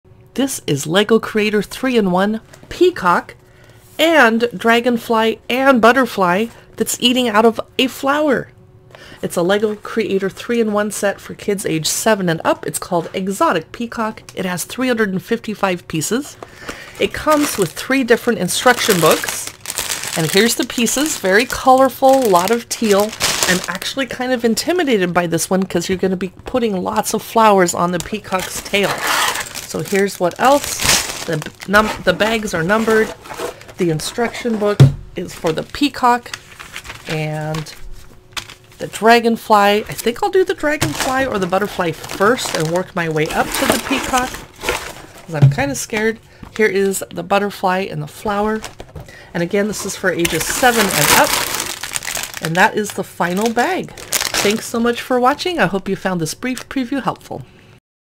LEGO 3-in-1 Exotic Peacock #31157 Dragonfly Butterfly
The LEGO 3-in-1 Exotic Peacock can also become a butterfly drinking from a flower, or a majestic dragonfly.

Visit our Amazon Storefront for more shoppable videos on books, toys, dolls, gourmet foods, kitchen items, tea and coffee.